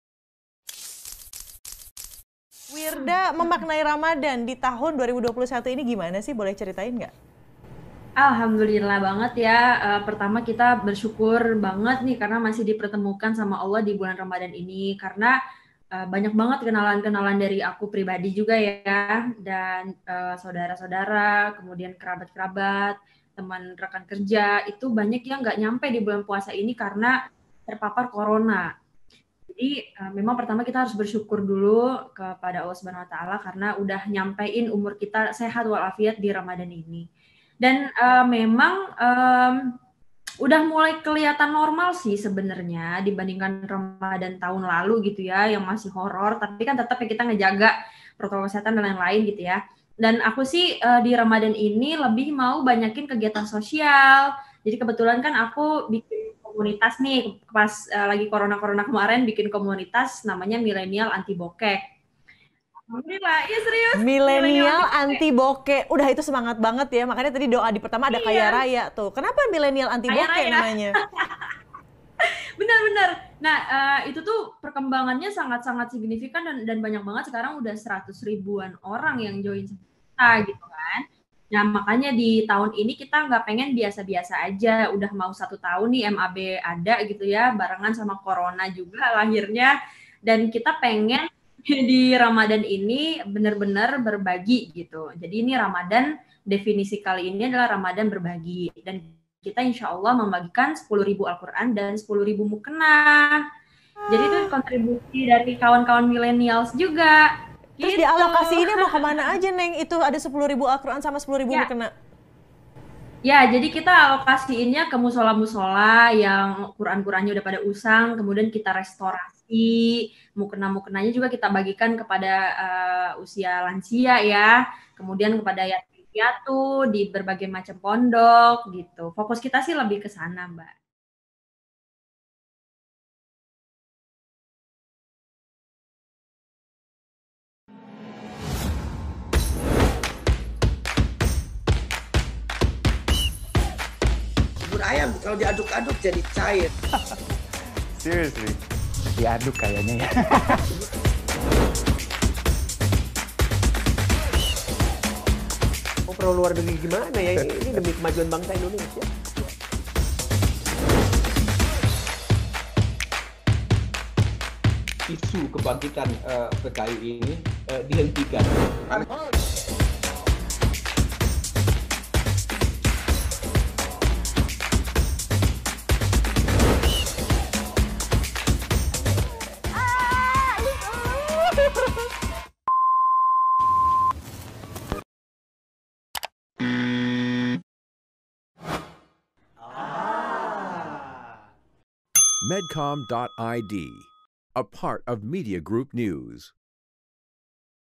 Wirda Mansur Perbanyak Kegiatan Sosial di Bulan Ramadan
Entrepreneur Wirda Mansur bersyukur masih dipertemukan dengan Bulan Ramadan kali ini. Wirda pun bercerita di Bulan Ramadan ini ia aktif dalam kegiatan sosial. Wirda membentuk komunitas Milenial Anti Bokek yang mengajak anak muda untuk berbagi.

Komunitas Milenial Anti Bokek ini sudah terbentuk selama satu tahun. Wirda ingin mengadakan kegiatan yang tidak biasa bersama komunitas Milenial Anti Bokek di Bulan Ramadan ini. Maka dari itu ia mengajak anggota komunitasnya untuk berbagi.